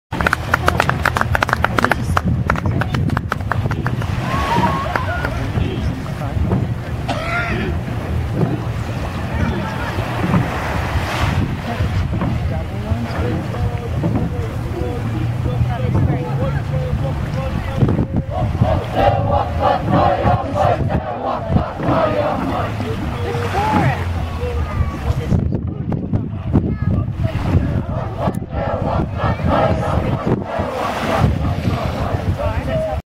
Waitangi Day - Waka
Dieses Video zeigt die Maori bei ihrer Show am Waitangi Day, neuseelands Nationalfeiertag, in ihren traditionellen Kanus (Waka).